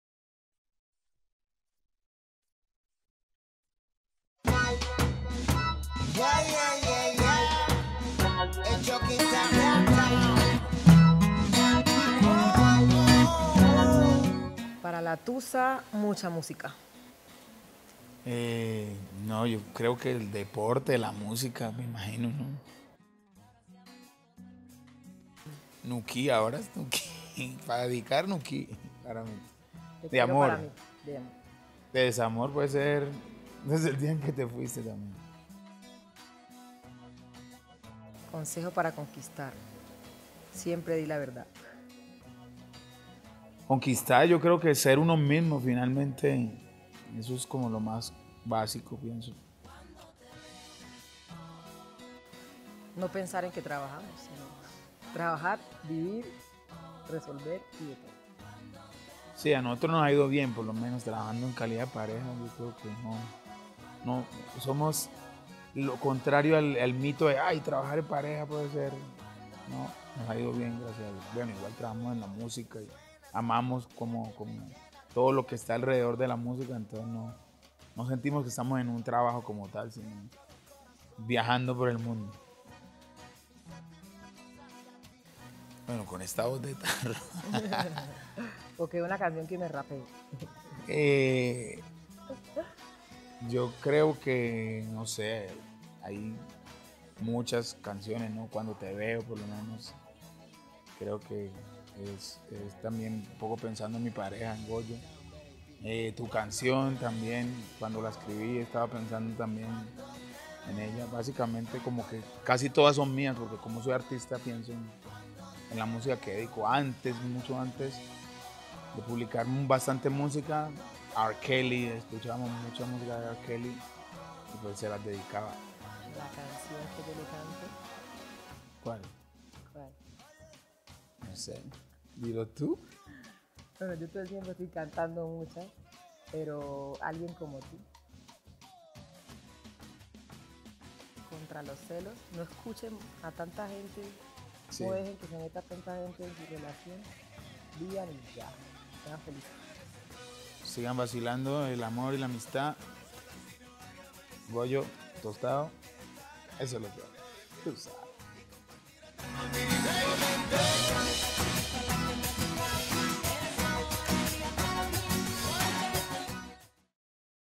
Consejos amor y amistad con Goyo y Tostao
Invitamos a Goyo y Tostao, de Chocquibtown, a que nos dieran sus consejos para mantenerse como pareja exitosa desde hace tantos años.